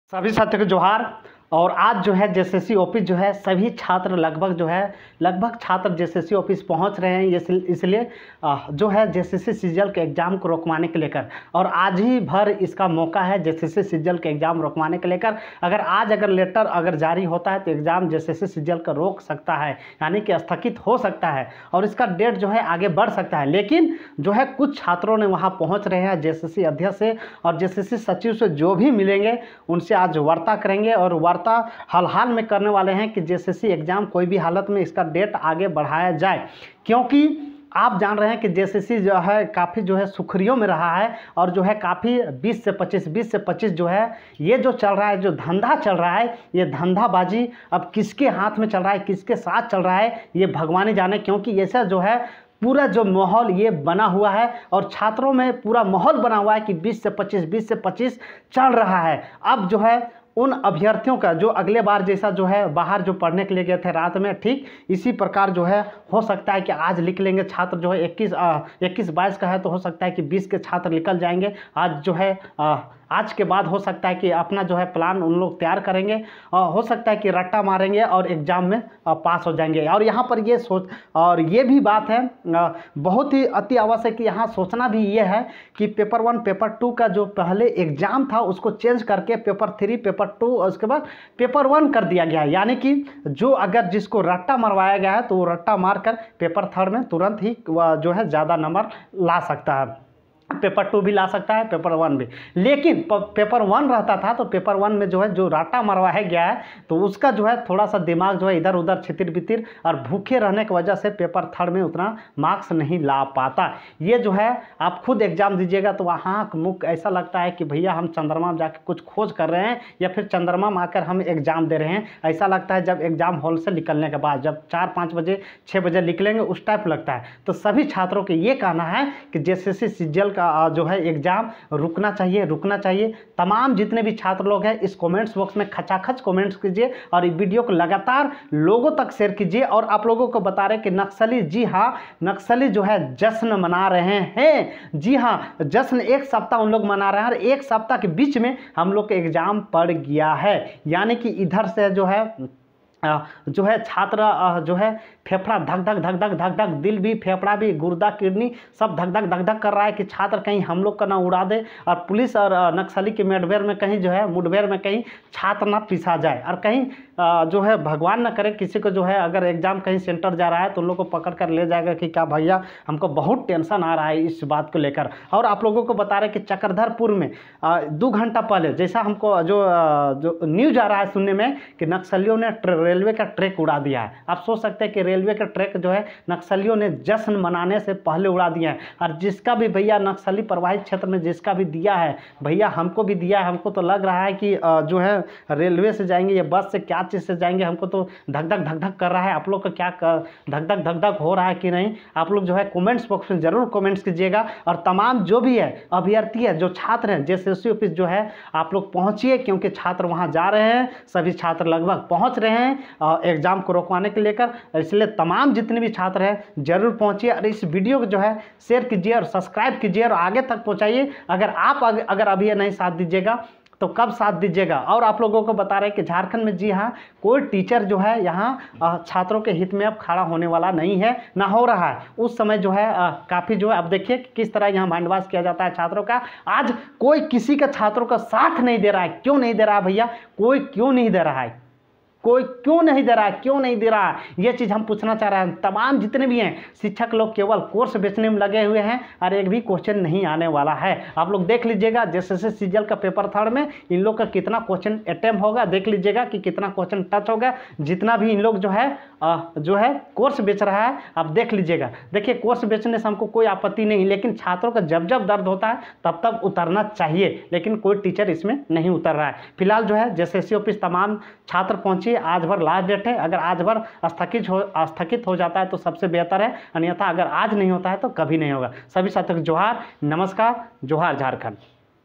JSSC CGL के छात्र JSSC ऑफिस जा रहें हैं // नक्सली से डर सता रहा Exam देने में युगेश_चंद्र _भारती
JSSC CGL Exam रोकवाने के लिए JSSC Office पहुंचे छात्र // नक्सली से डर सता रहा है कही उड़ा ना दे 😢